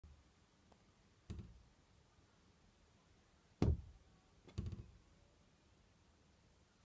Poking a Door
Today I am poking a door.